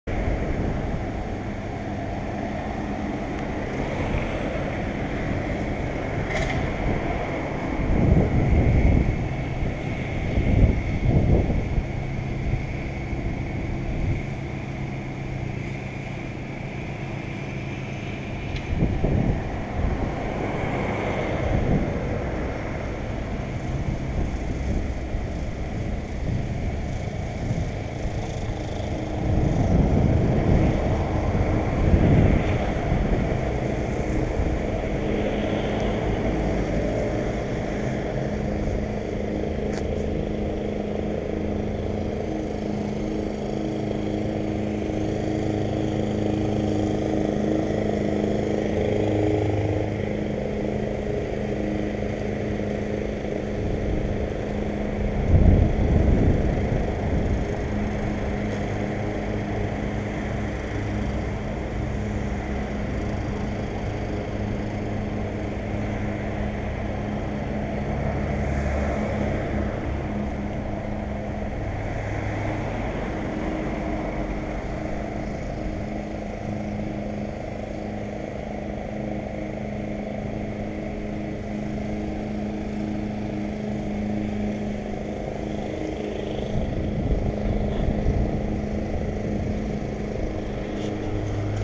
Acabado de chegar e vista geral da Paragem do Mouchão
PT - Quarto video, gravado com uma GoPro Hero, em 720p 50fps.

EN - Just arrived and general view of the Mouchão Stop

Fourth video, recorded whit a GoPro Hero, in 720p 50fps.